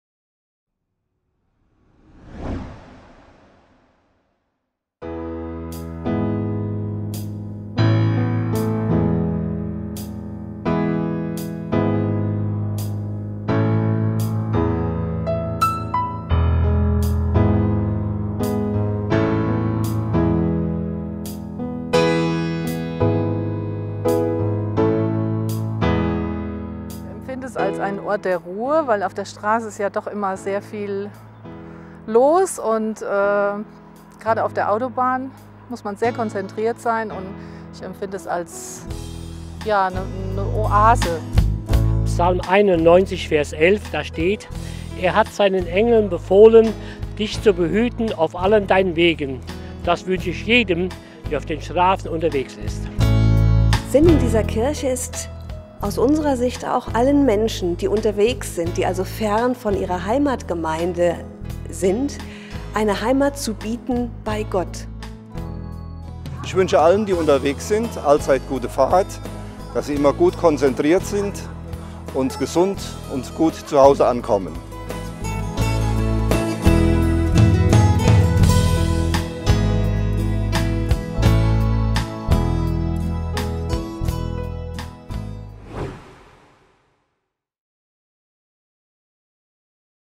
Ein Rastplatz für die Seele!
Wie eine Oase mitten im Verkehrstrubel ist die Autobahnkirche Siegerland an der A 45. Doch sie will noch mehr sein.